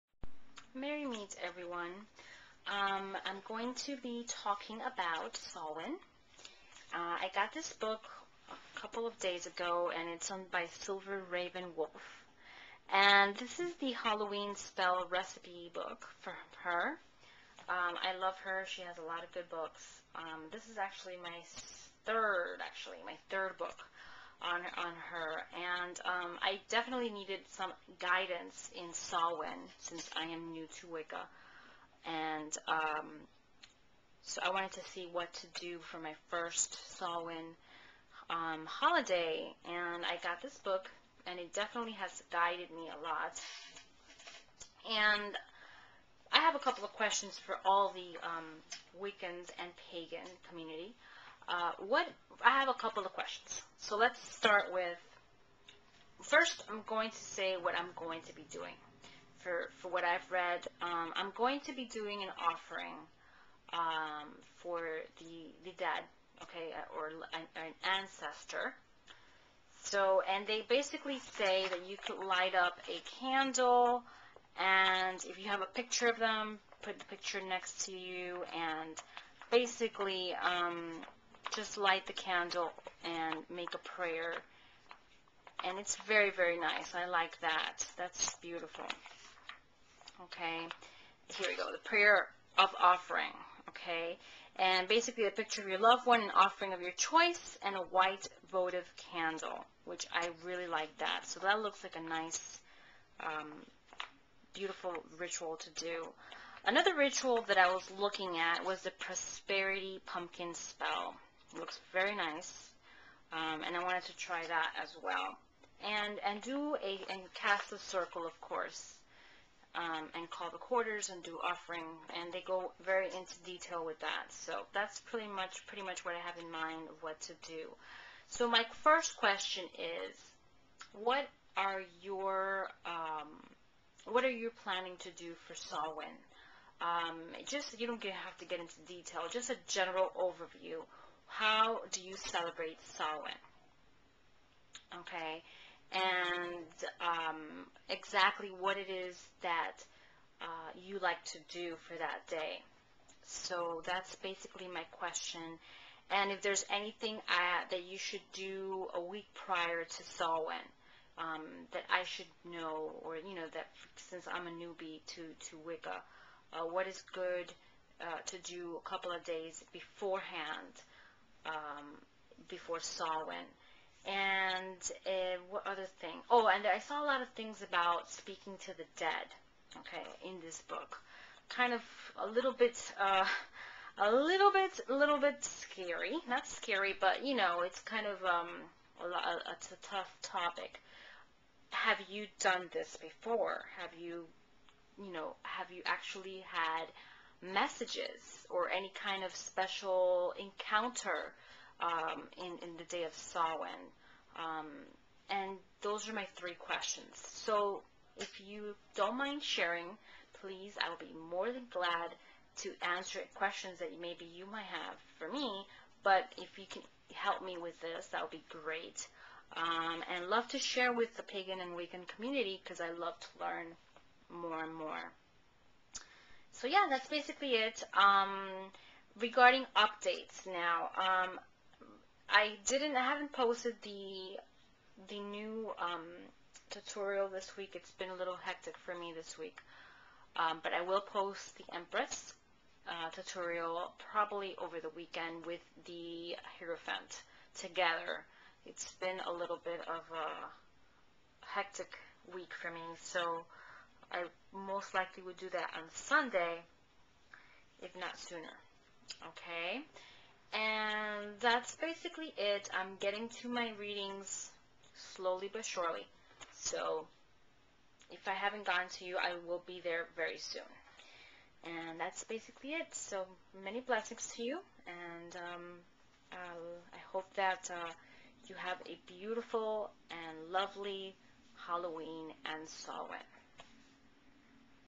Samhain Holiday & Reading Updates
My plans for Samhain:
-Cast Circle
-Prayer offering for Ancestor
-Prosperity Pumpkin Spell

My 3 questions:
1. How did you celebrate Samhain?

2. What should I do a week prior Samhain?

3. Have you had any supernatural encounter such as receiving messages from ancestors etc.,?Anything special that has happened to you on this special holiday?

4. Didn't post this question, Anything good you recommend for this day?

Empress Tutorial will posted over the weekend!

Happy Holidays!!!